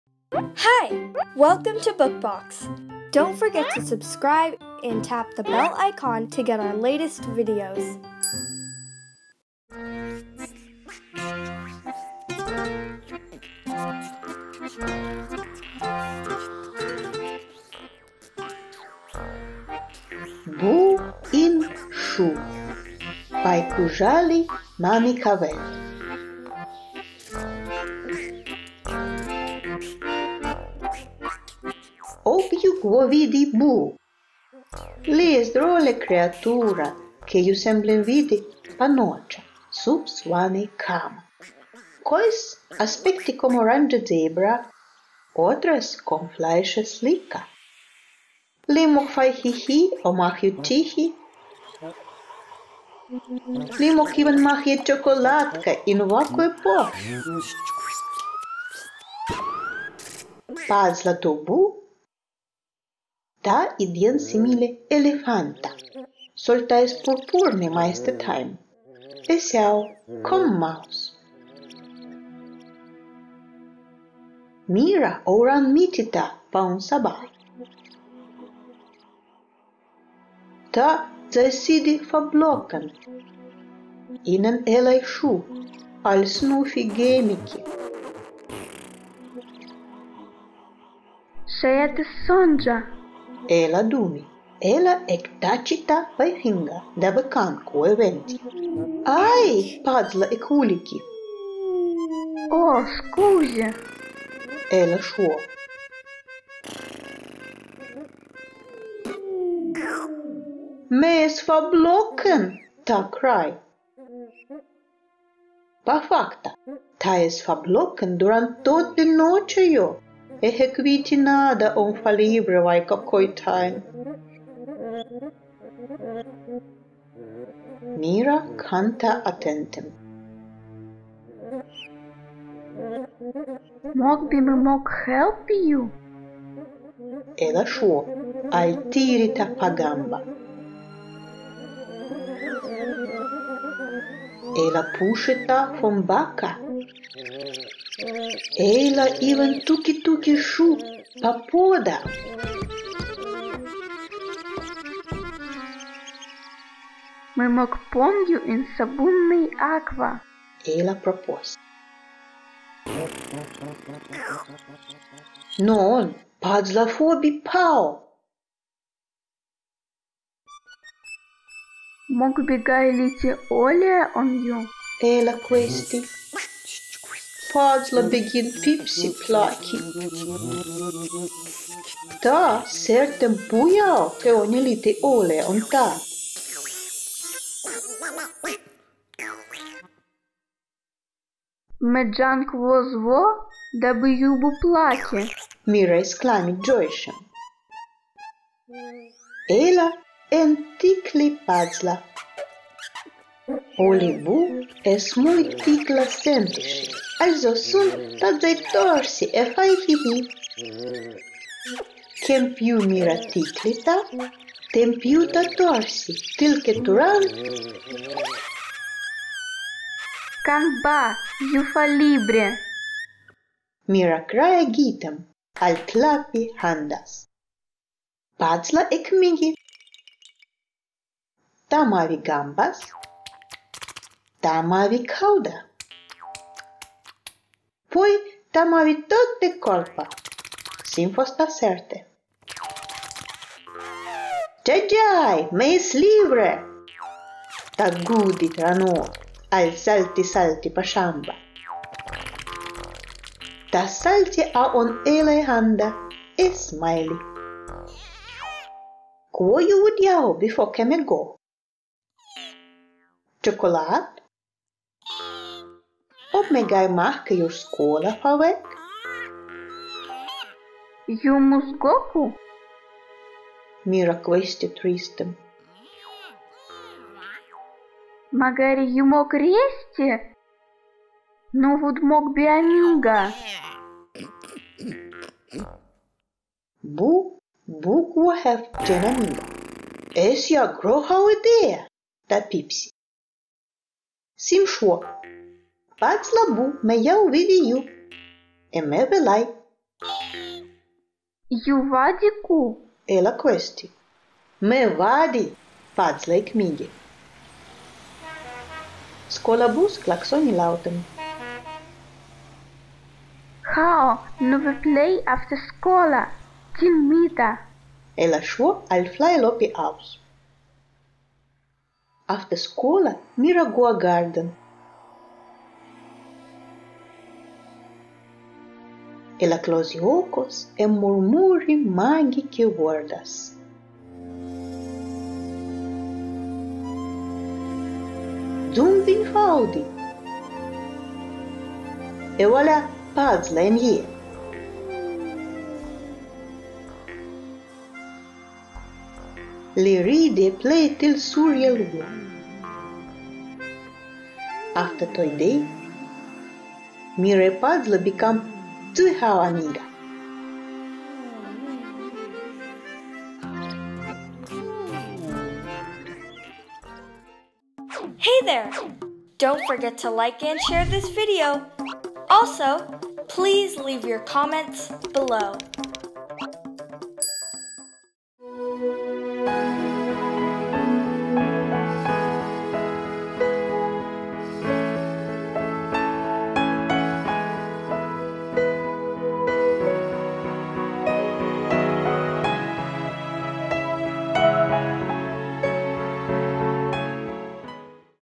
Buu in shu : Lerni Lidepla kun subtitla. - Historia fo kinda e adulta "BookBox.com"
Boos are playful creatures. Meera stumbles upon Puzzle, the Boo, stuck inside her shoe.

Buu in shu
Bay Kuzhali Manikavel
Ob yu gwo vidi Buu?
Li es drole kreatura
ke yu semblem vidi
pa nocha sub swa-ney kama.
Koys aspekti kom
oranje zebra,
otres kom flai-she slika.
Li mog fai hi-hi
o mah yu chihi,
li mog iven
mah-ye chokolatka
in vakue posh!
Pazla to Buu,
ta idyen simili
elefanta,
sol ta es purpur-ney
maiste taim
e syao kom maus.
Mira ouran
miti ta pa un sabah.
Ta zai sidi fa-bloken
inen elay shu,
al snufi-gemi-ki.
“Shayad es sonja,”
ela dumi.
Ela ek-tachi ta bay finga
dabe kan kwo eventi.
“Ay!” Pazla ek-wuli-ki.
“Oo, skusi,” ela shwo.
“Me es fa-bloken!” ta krai.
Pa fakta,
ta es fa-bloken
duran tote nocha yo
e he kwiti nada
om fa-libre vaika koytaim.
Mira kan ta atentem.
“Mogbi me mog helpi yu,”
ela shwo
al tiri ta pa gamba.
Ela pushi ta fon baka.
Ela iven tuki-tuki shu
pa poda.
“Me mog pon yu
in sabun-ney akwa!”
ela proposi.
Non,
Pazla fobi pao.
“Mogbi gai liti olea on yu?”
ela kwesti.
Pazla begin pipsi-plaki.
Ta sertem bu yao
ke oni liti olea on ta!
“Me jan kwo zwo
dabe yu bu plaki!”
Mira exklami joi-shem.
Ela en-tikli Pazla.
Oli Buu es
muy tikla-sentishil,
also sun
ta zai torsi e fai hi-hi.
Kem pyu Mira tikli ta,
tem pyu ta torsi,
til ke turan...
“Kan ba! Yu fa-libre!”
Mira krai agitem
al klapi handas.
Pazla ek-migi.
Ta mavi gambas.
Ta mavi kauda.
Poy ta mavi tote korpa,
sim fo sta serte.
“Jay-jay! Me es libre!”
ta gudi tra nos
al salti-salti pa shamba.
Ta salti a on elay handa
e smaili.
“Kwo yu wud yao
bifoo ke me go?
Chokolat?
Ob me gai
mah ke yur skola fa-wek?”
“Yu mus go ku?”
Mira kwesti tristem.
“Magari yu mog resti.
Nu wud mog bi amiga!”
Buu bu gwo hev jen-amiga.
“Es ya gro-hao idea!”
ta pipsi.
“Sim shwo
Pazla Buu,
me yao vidi yu',
e me ve lai!”
“Yu wadi ku?”
ela kwesti.
“Me wadi!”
Pazla ek-migi.
Skola-bus klaxoni lautem.
“Hao, nu ve plei afte skola.
Til mita!”
ela shwo
al flai-lopi aus.
Afte skola
Mira go a garden.
Ela klosi okos
e murmuri
magike wordas.
Zumbing fa-audi,
ewalaa Pazla en-ye!
Li ridi e plei
til suryalwo.
Afte toy dey
Mira e Pazla
bikam zuy hao amiga.

Illustration : Emanuele Scanziani
Animation : Alfrin Multimedia
Translation : Anastasia Lysenko
Narration : Anastasia Lysenko
Want to participate in a volunteer driven activity at BookBox,Add My Language.